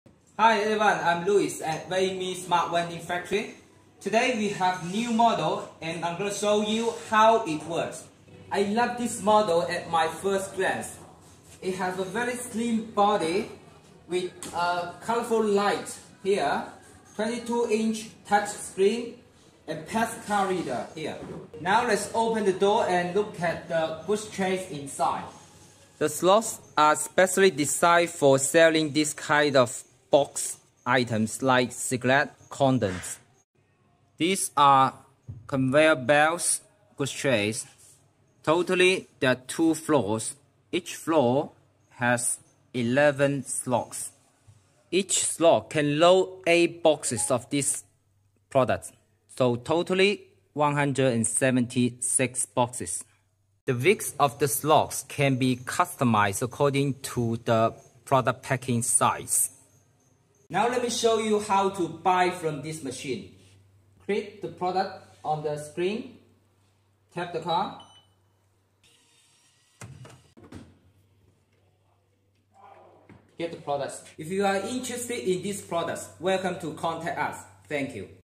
Our wall mounted Cigarette vape vending machine detail introduction
Our wall mounted Cigarette vending machine detail introduction

Our wall mounted Cigarette vape vending machine detail introduction - YouTube

Transcript:
(00:00) hi everyone I'm Luis at by me smart one in Factory today we have new model and I'm going to show you how it works I love this model at my first class it has a very slim body with a colorful light here 22 inch touchscreen and test car reader here now let's open the door and look at the good trays inside the slots are specially designed for selling this kind of box items like cigarette content these are conveyor belts good trace totally there are two floors each floor has 11 slots each slot can load eight boxes of this
(00:48) product so totally 176 boxes the weeks of the slots can be customized according to the product packing size now let me show you how to buy from this machine create the product on the screen tap the car get the products if you are interested in this product welcome to contact us thank you